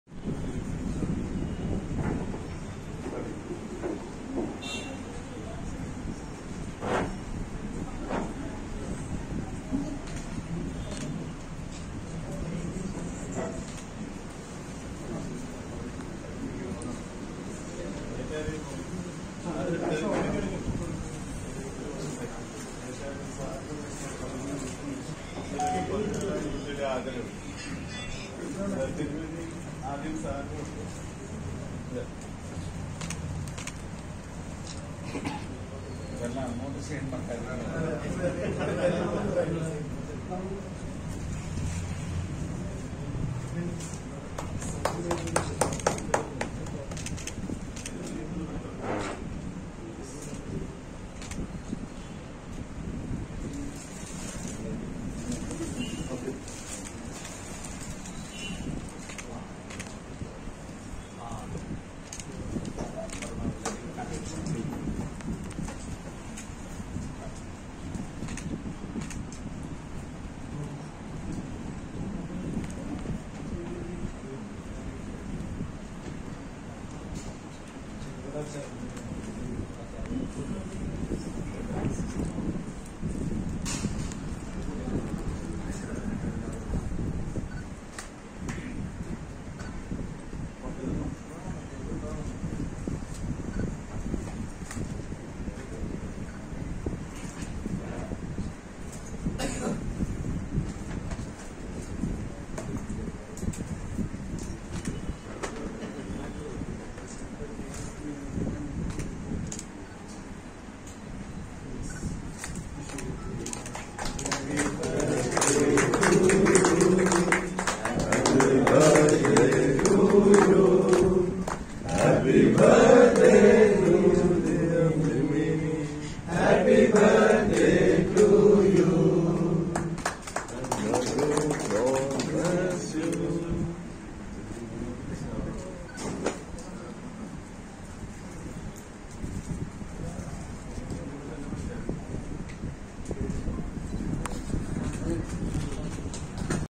കേന്ദ്ര മന്ത്രി സുരേഷ് ഗോപി തിരുമേനിയുടെ ബർത്ത് ഡേ ആഘോഷം ആക്കി
central minister Sri. Suresh Gopi Wishes Dr.. Mar Aperem Metropolitan On his 84th Bairthday At Metropolitan s Palace